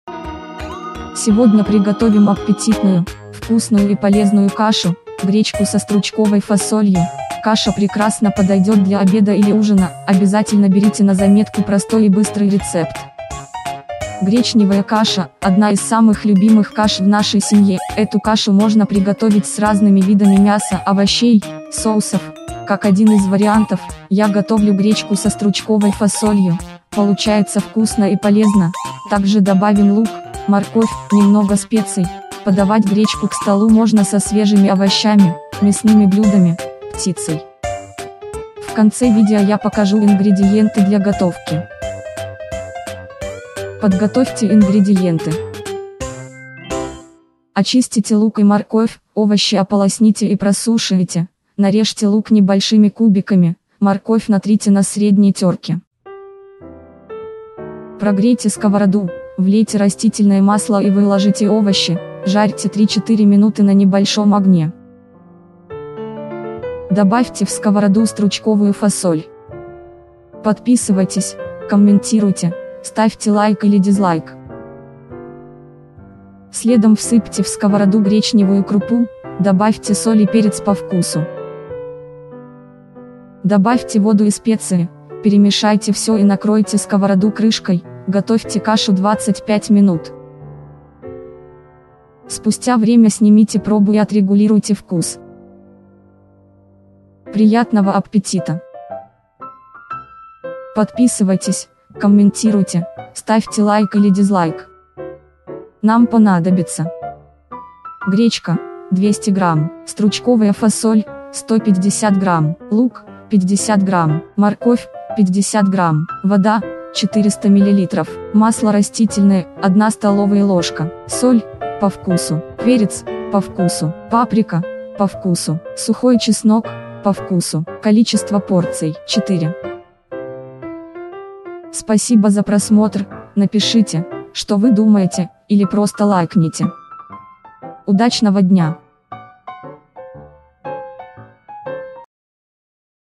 ★ВКУСНЕЙШИЙ И ОЧЕНЬ КРАСИВЫЙ РЕЦЕПТ! ★ ГРЕЧКА СО СТРУЧКОВОЙ ФАСОЛЬЮ ★
★Вкус - божественный● Сегодня приготовим аппетитную, вкусную и полезную кашу — гречку со стручковой фасолью● Каша прекрасно подойдет для обеда или ужина● Обязательно берите на заметку простой и быстрый рецепт!